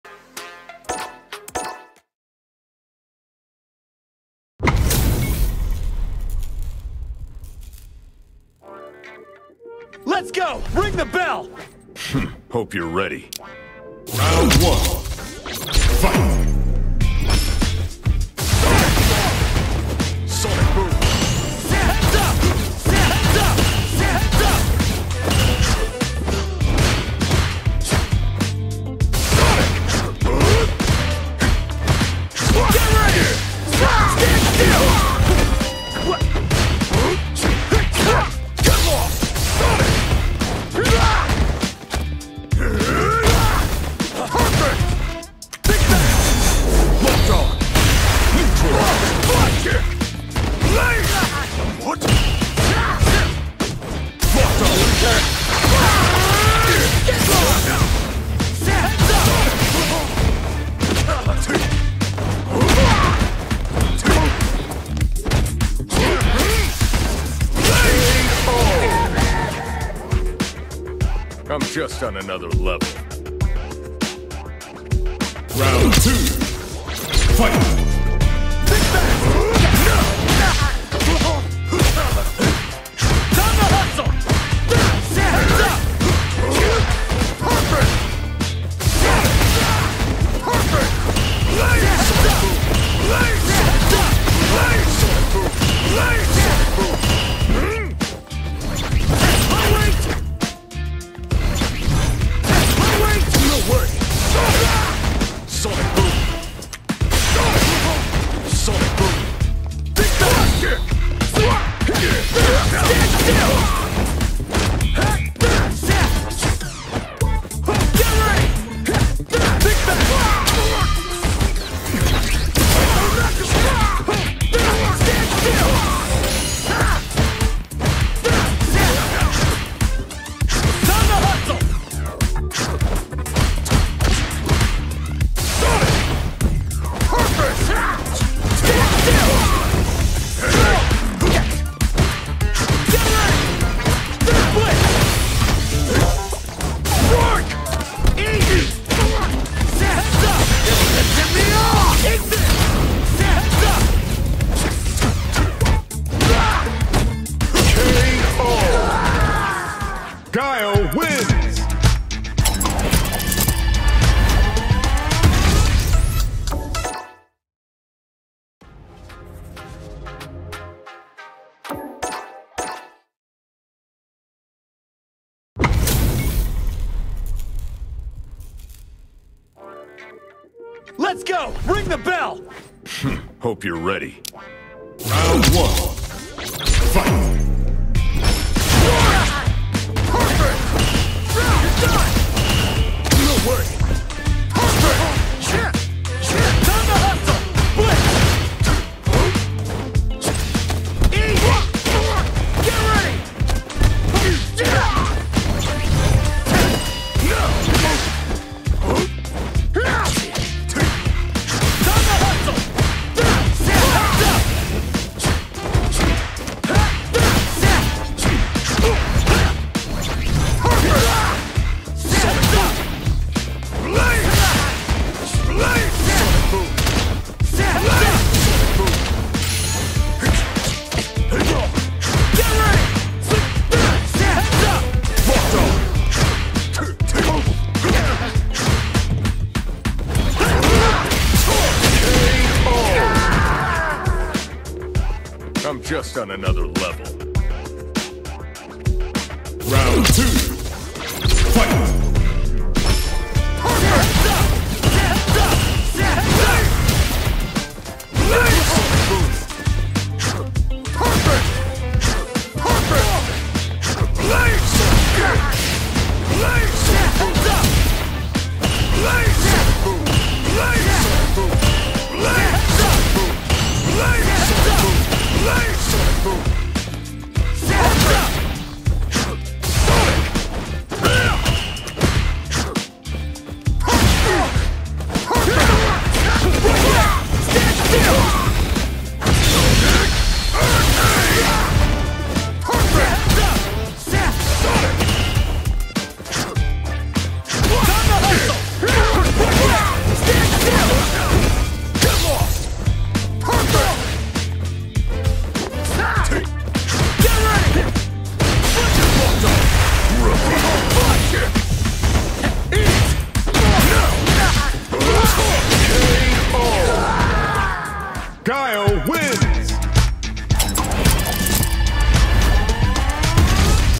Guile Master rank up
11th character in master. Too bad it was a super laggy match for the rank up.